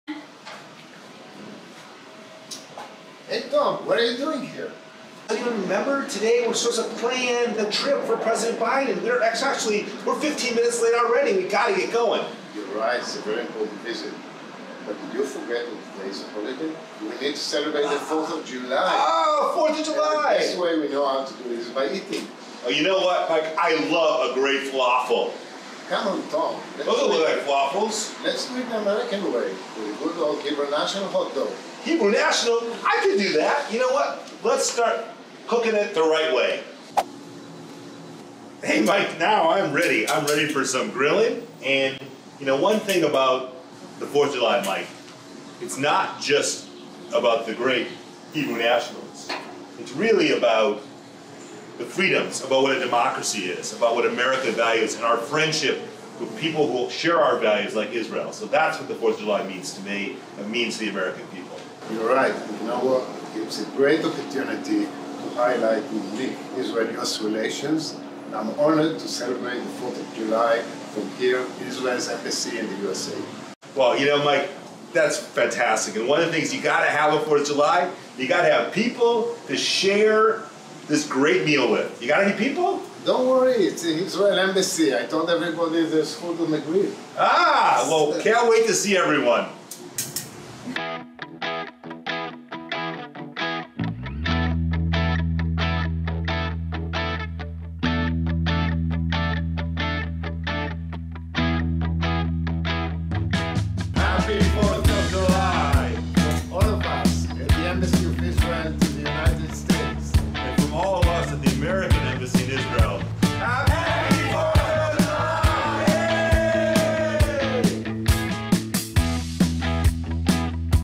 Happy 4th of July America!
Ambassador Michael Herzog & Ambassador Tom Nides celebrate the 4thofjuly at the Embassy, the Israeli-American way. Wishing everyone a happy 4th of July! 🇺🇸🇮🇱🎉